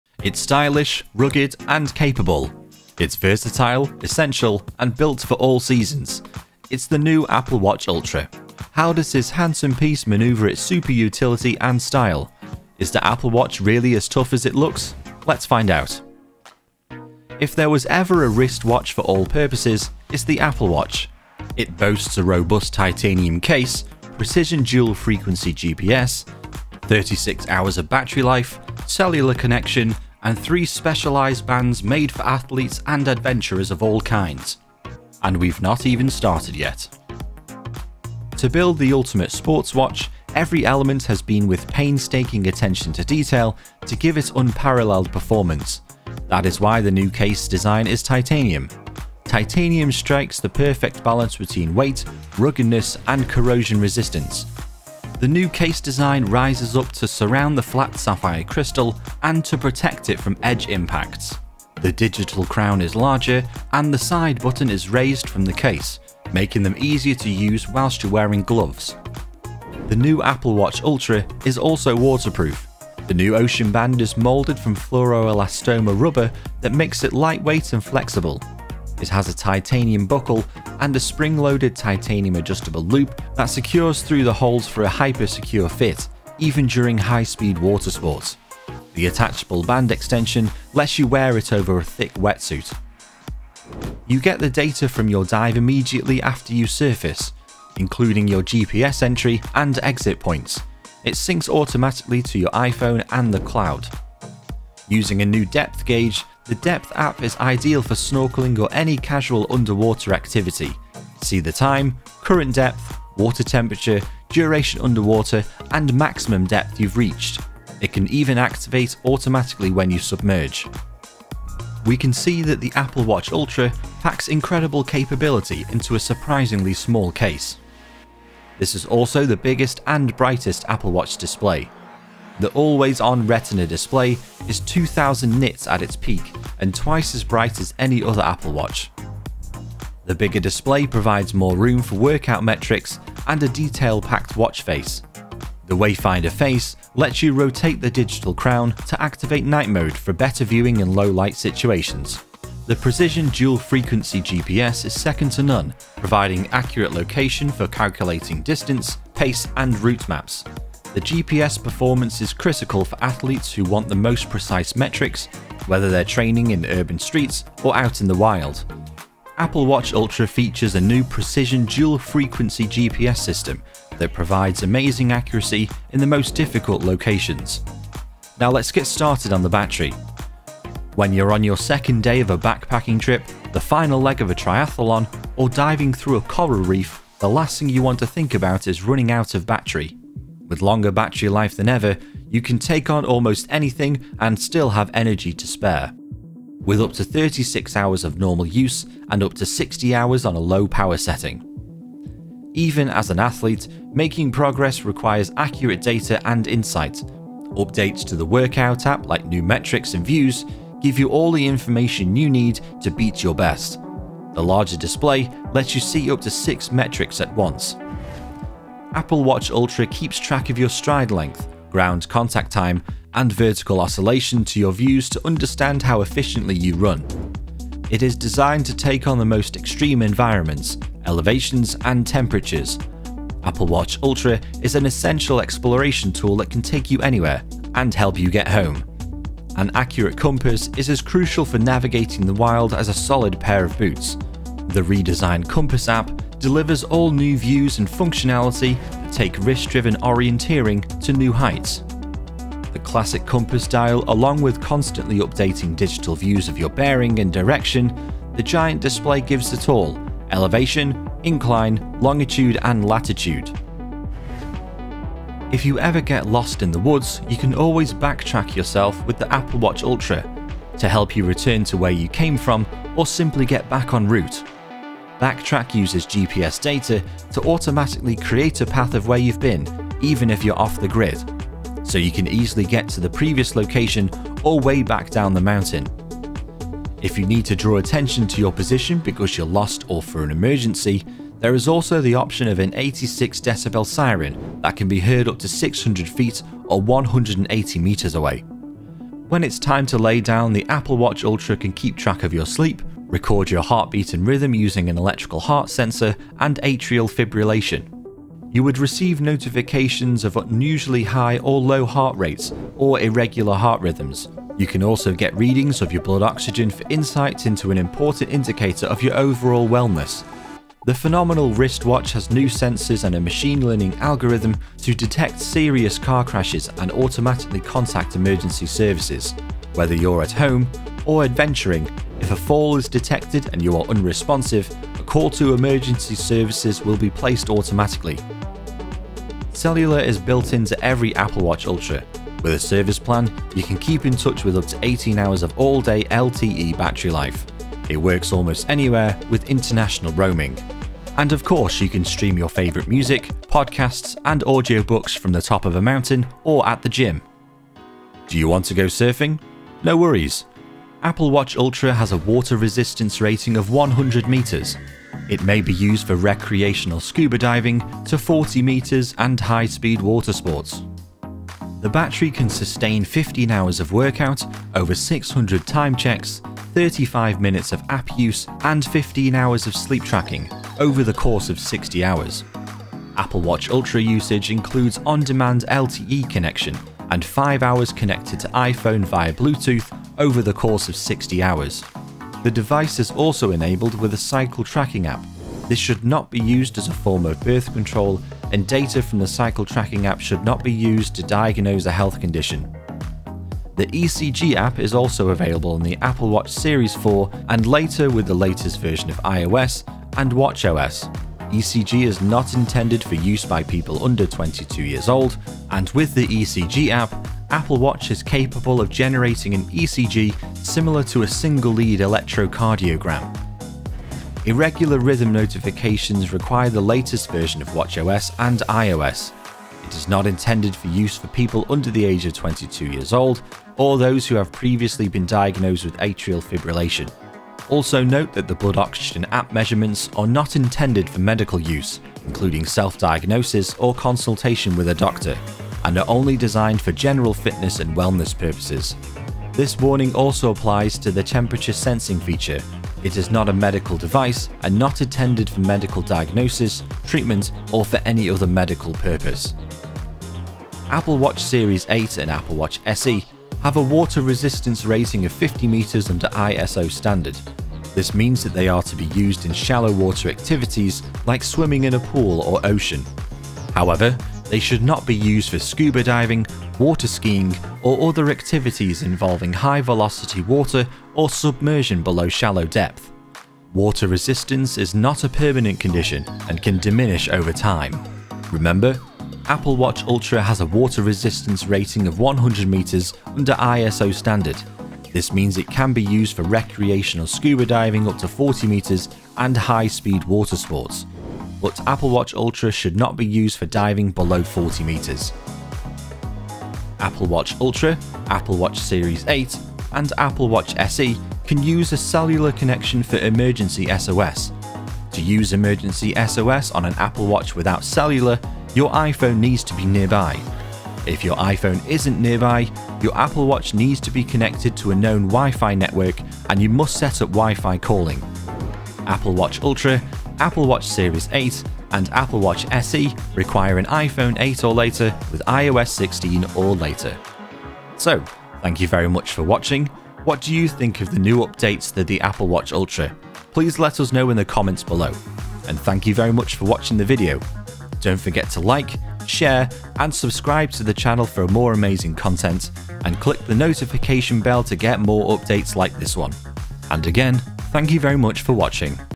Apple Watch Ultra Will Change Your Life FOREVER
It's stylish, rugged and capable. It's versatile, essential and built for all seasons. It's the Apple Watch Ultra! How does this handsome piece maneuver its super utility and style? Is the applewatch Ultra really as tough as it looks? Keep watching to find out!

About Tech Empire
🎥 Videos about Technology, Future and Peripherals
🎨 Written, voiced and produced by Tech Empire